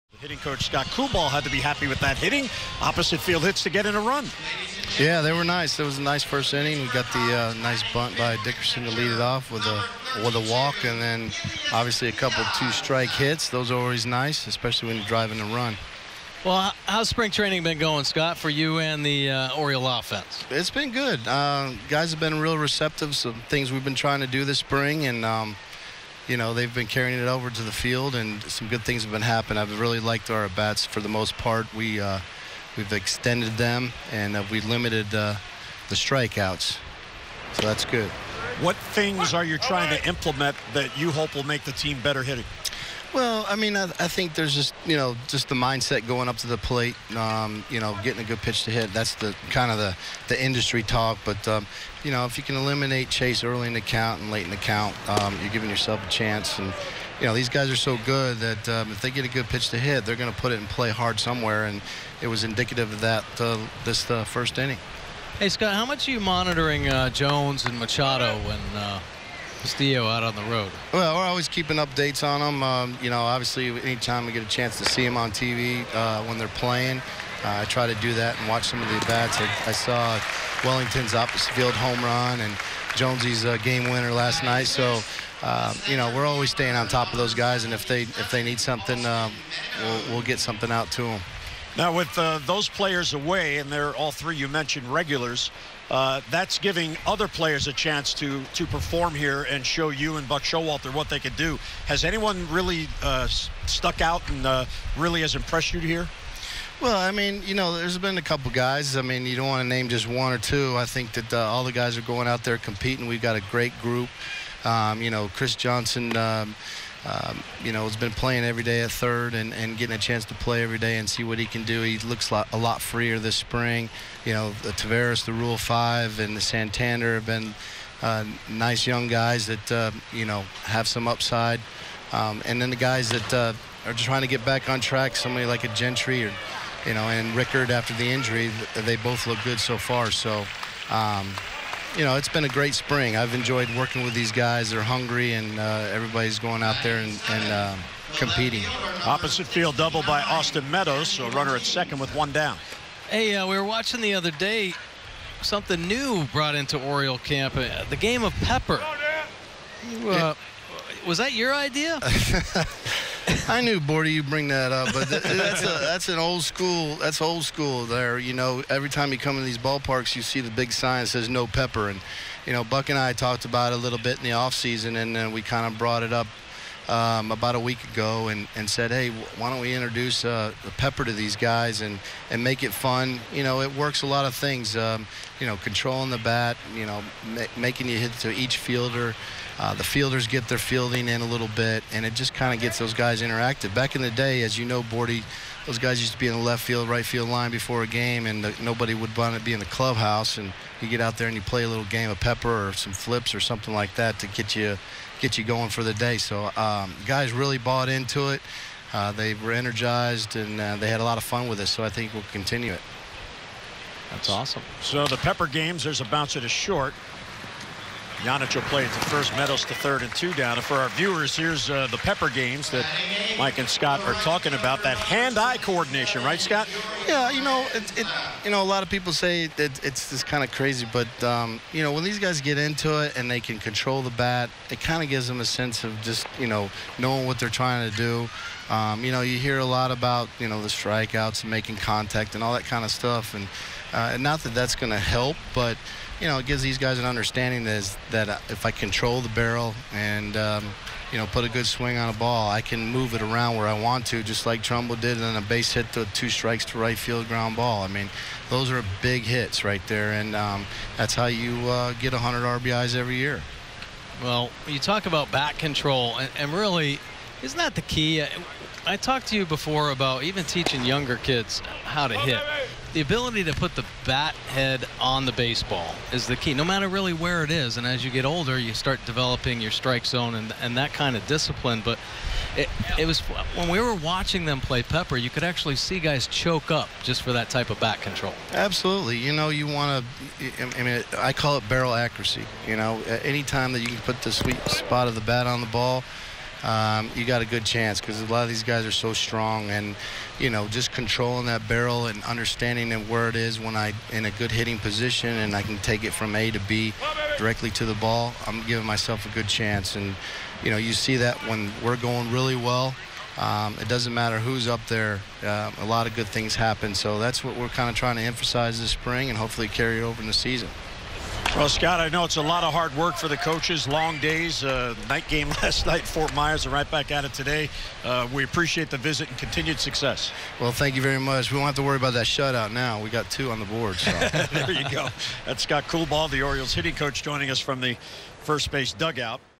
Scott Coolbaugh on Orioles playing "Pepper" in spring training
Orioles hitting coach Scott Coolbaugh talks about the Orioles playing the often banned game "Pepper" in spring training.